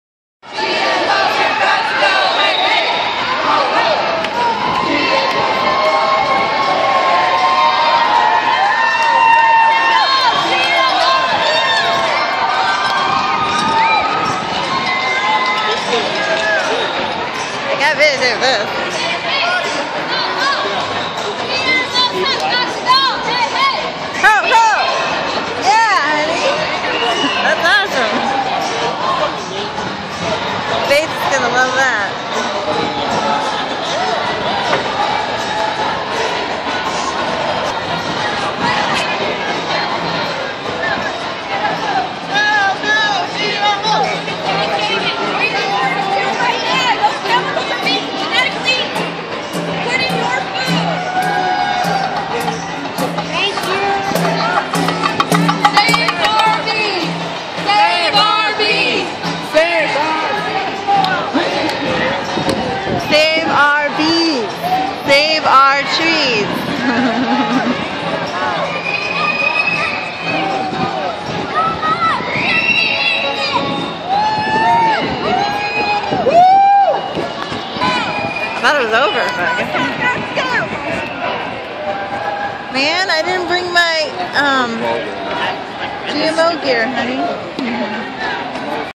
March against Monsanto las vegas
While we were in vegas we got to see this awesome march! It was pretty awesome how everyone came together for this